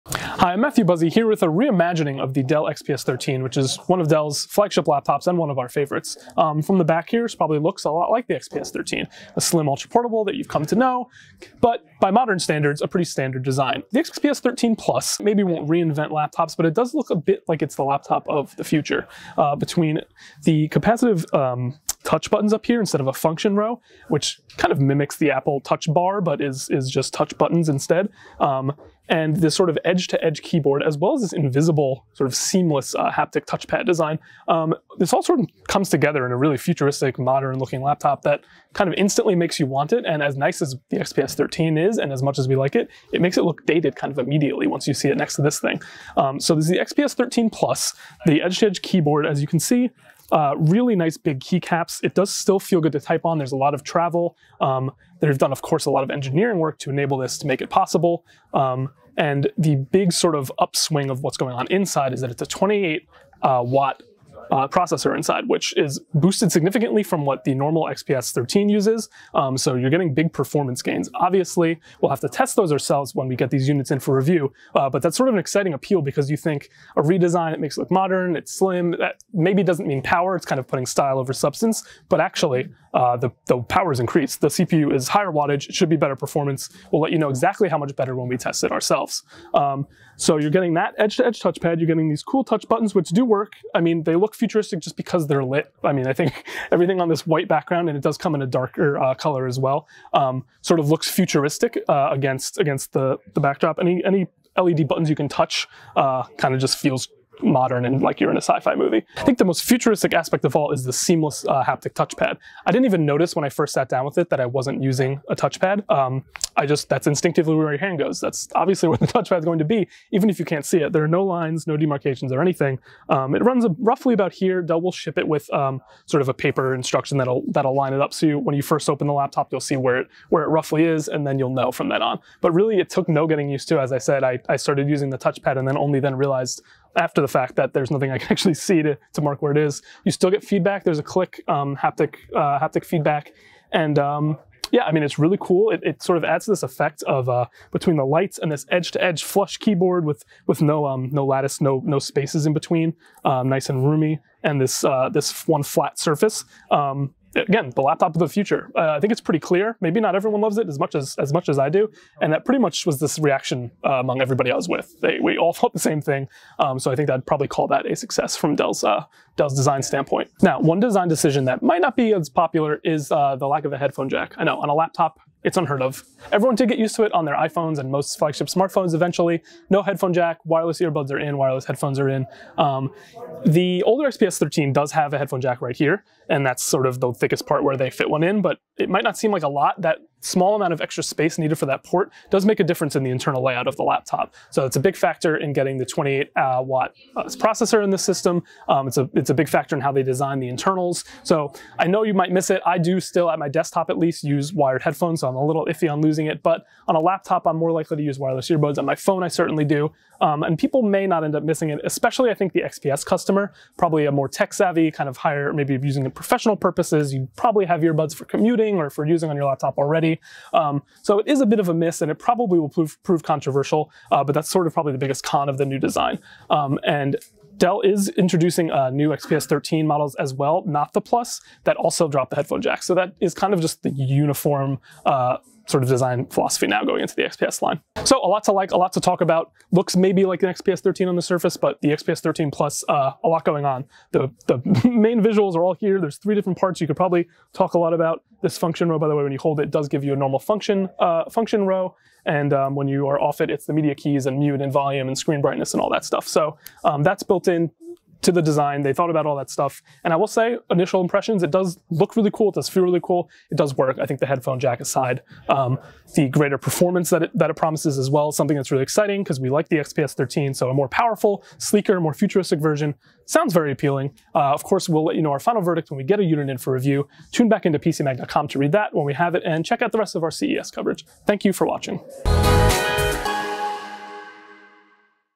Dell XPS 13 Plus Hands-On: The Laptop of the Future is Here Now
The new-for-2022 spinoff of Dell's flagship ultraportable laptop is a real head-turner.